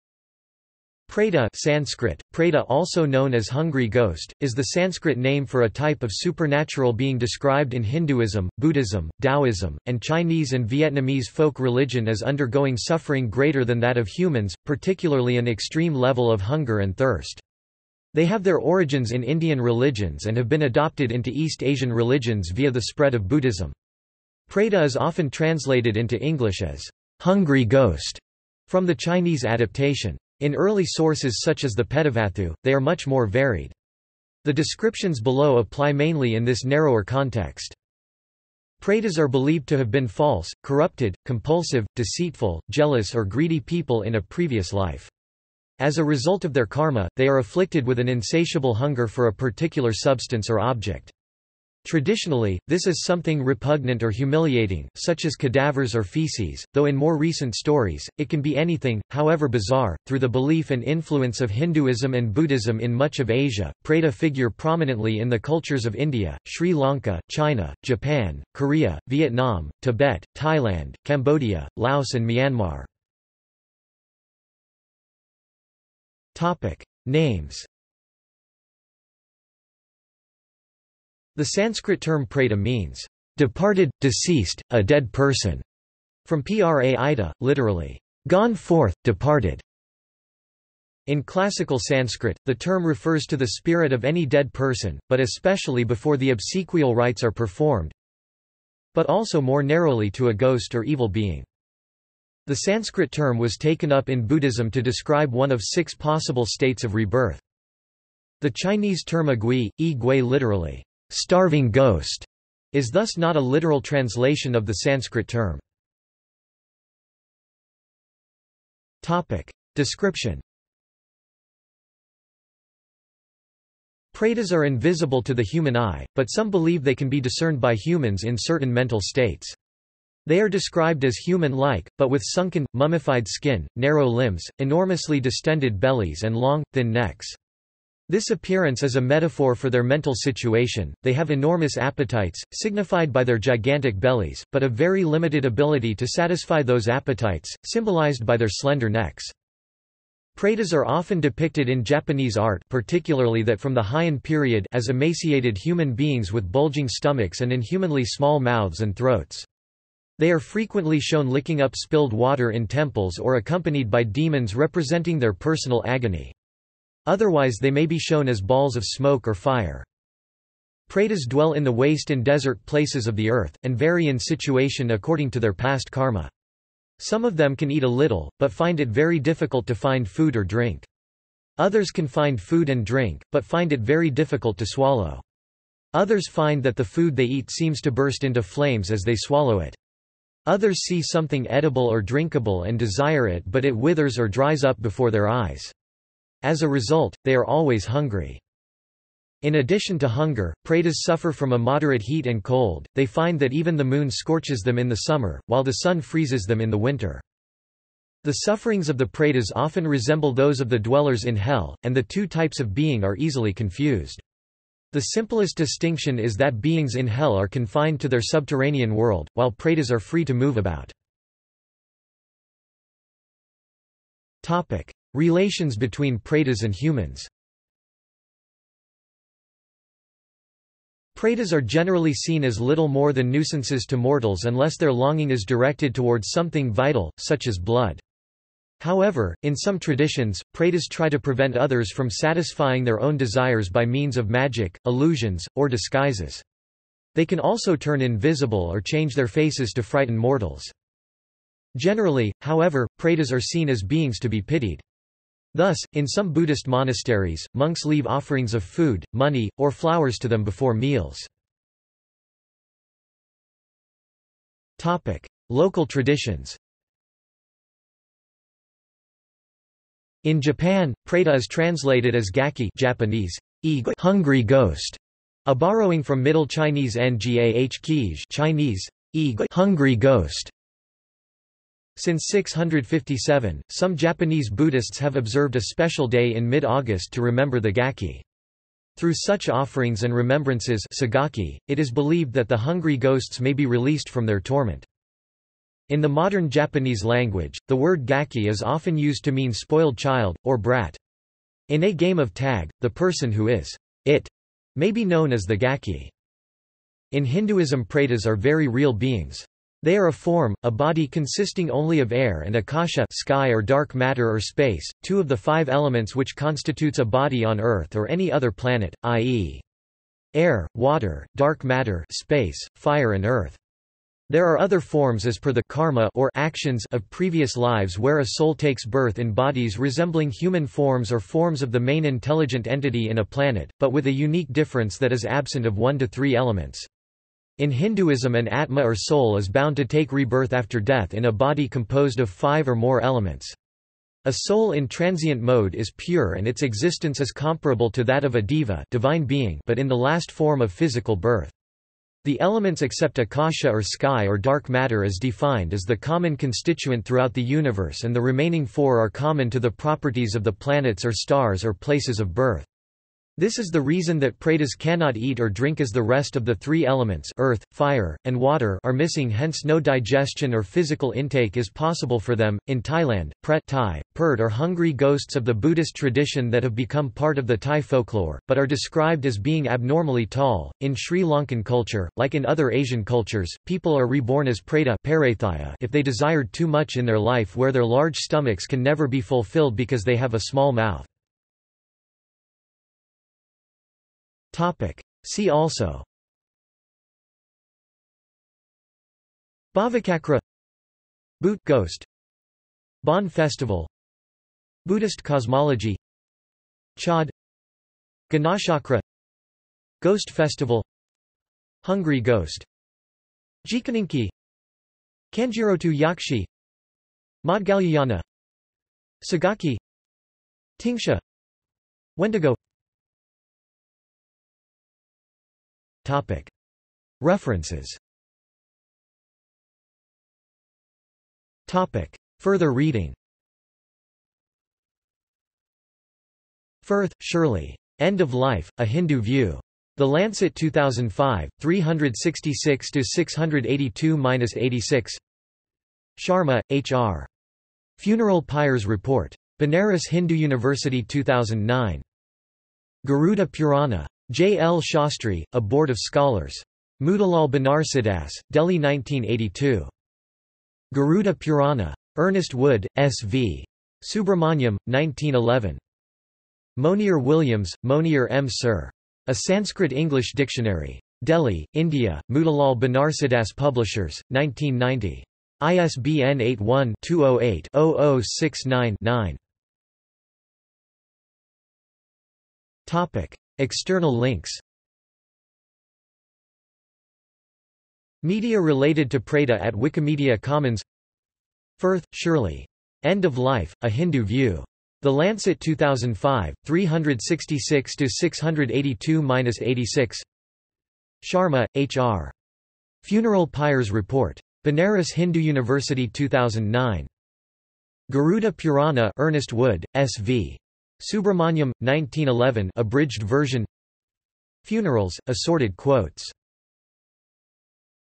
Preta | Wikipedia audio article
This is an audio version of the Wikipedia Article:
Preta

- Socrates

SUMMARY
Preta (Sanskrit: प्रेत), also known as hungry ghost, is the Sanskrit name for a type of supernatural being described in Hinduism, Buddhism, Taoism, and Chinese and Vietnamese folk religion as undergoing suffering greater than that of humans, particularly an extreme level of hunger and thirst. They have their origins in Indian religions and have been adopted into East Asian religions via the spread of Buddhism. Preta is often translated into English as "hungry ghost" from the Chinese adaptation. In early sources such as the Petavatthu, they are much more varied. The descriptions below apply mainly in this narrower context.
Pretas are believed to have been false, corrupted, compulsive, deceitful, jealous or greedy people in a previous life. As a result of their karma, they are afflicted with an insatiable hunger for a particular substance or object. Traditionally, this is something repugnant or humiliating, such as cadavers or feces, though in more recent stories, it can be anything, however bizarre.Through the belief and influence of Hinduism and Buddhism in much of Asia, preta figure prominently in the cultures of India, Sri Lanka, China, Japan, Korea, Vietnam, Tibet, Thailand, Cambodia, Laos and Myanmar.